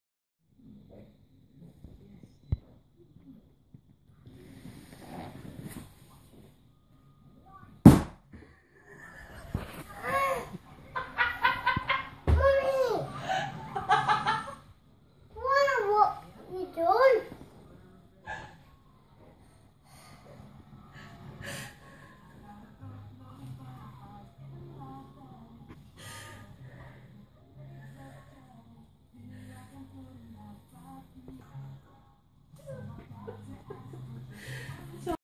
Balloon seat goes pop
I warn my son that the balloon will pop... He never listens .